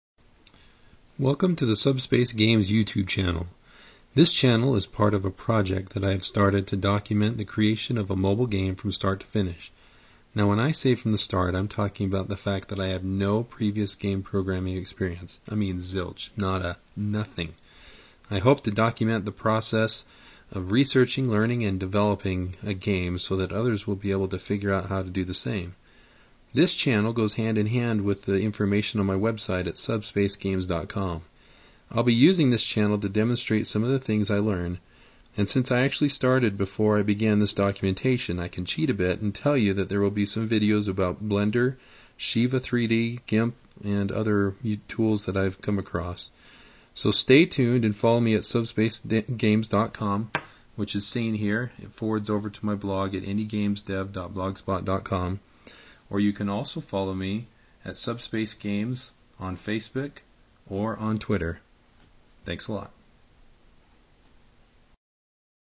Indie Game Project Intro
Introduction to my independent game development project series.  I will be developing a mobile game from scratch, knowing next to nothing about the game development process.  Check out my blog at subspacegames.com or indiegamesdev.blogspot.com.  Also follow Subspace Games on Facebook and SubspaceGames on Twitter.